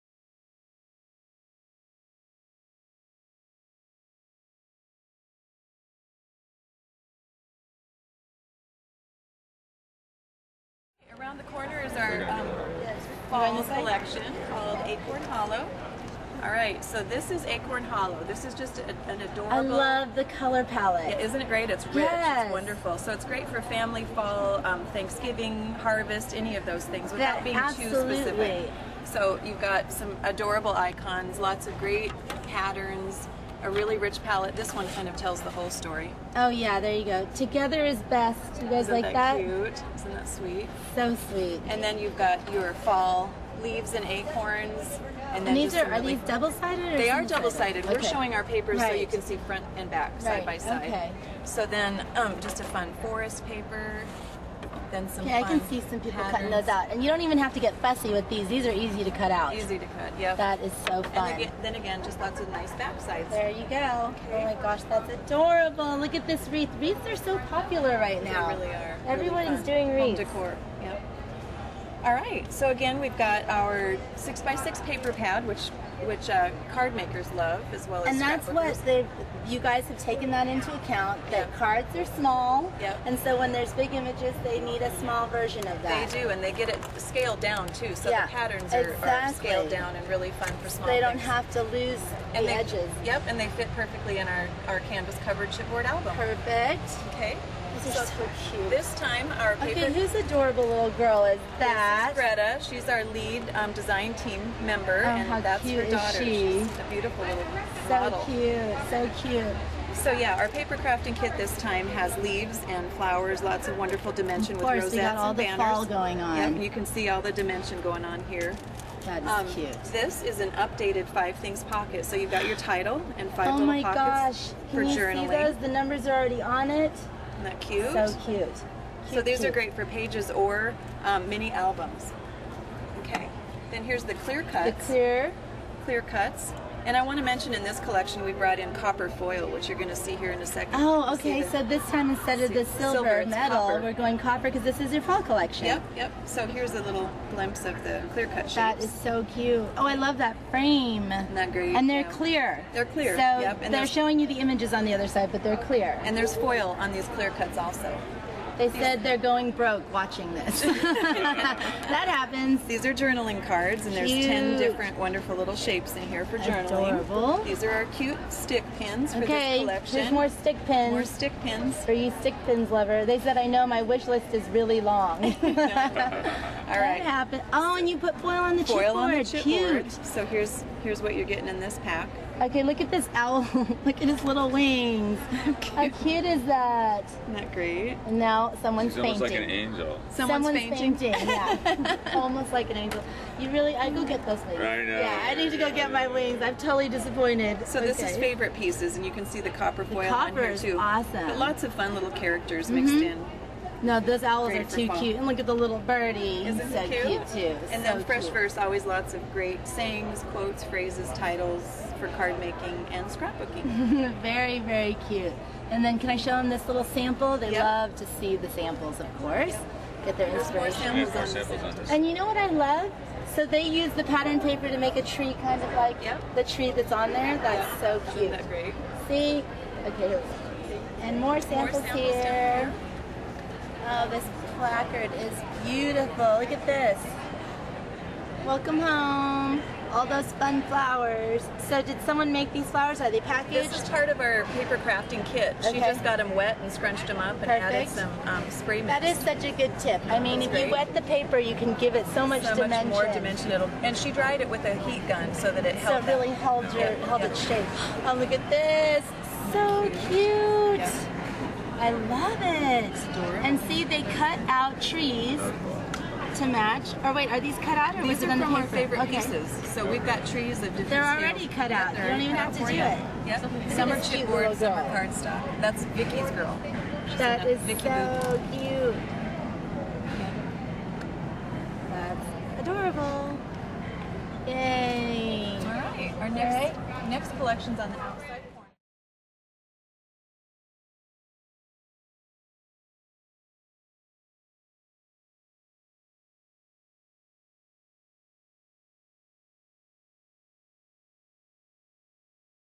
Little Yellow Bicycle Reveals Acorn Hollow on LIVE from CHA with Charity Wings
Elena Lai Etcheverry from Charity Wings aka Scrapbook Royalty interviews manufacturers and designers at the Craft & Hobby Association Trade show in Chicago as part of an online streaming event called LIVE from CHA.  The event was held to benefit A Place to Bark Bernie, Berlin's amazing animal rescue.  Here is Elena with Sharon Ann Kropp, Senior Creative Director and Ross Zachs from Little Yellow Bicycle showing the new Acorn Hollow line just released at CHA!  to learn all about this and many other amazing charitable events.